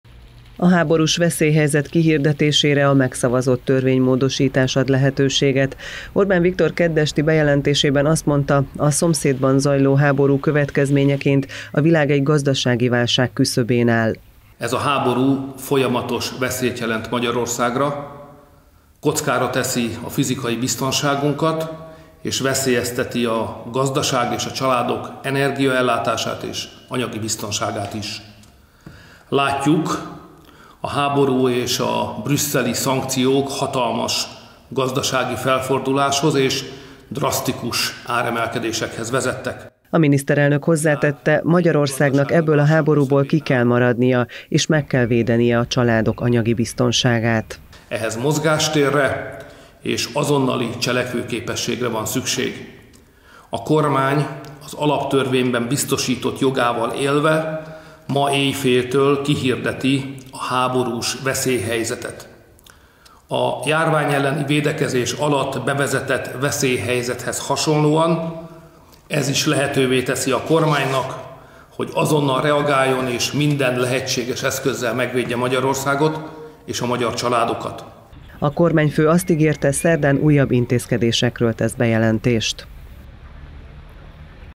HÁBORÚS VESZÉLYHELYZET
Háborús veszélyhelyzet lépett életbe éjfélkor. Az alaptörvény tizedik módosítását is elfogadta kedden az Országgyűlés, ezt követően jelentette be Orbán Viktor, hogy éjféltől kihirdeti a háborús veszélyhelyzetet.